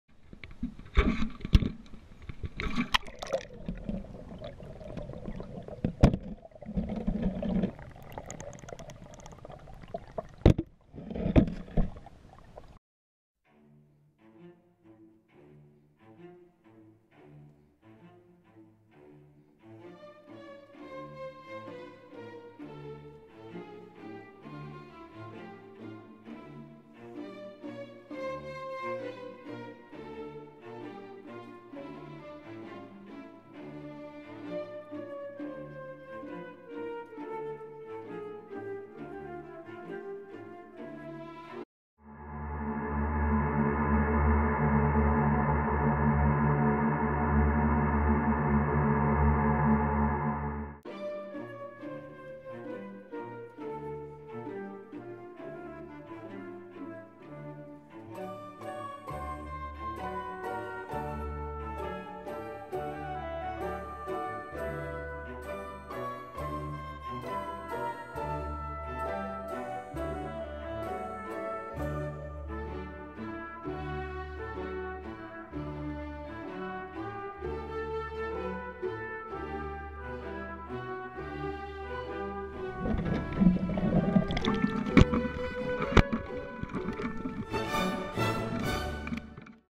GoPro in a Minnow Trap! Minnow & Water Snake Footage!
Have you ever wondered what it was like to see how minnows get trapped?  Check this out!  Watch the Water Snake chasing around the minnows!